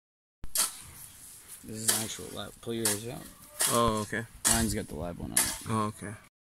The crew working together as a team to finishing tilling(1)
Working in the eSwatini son. 2021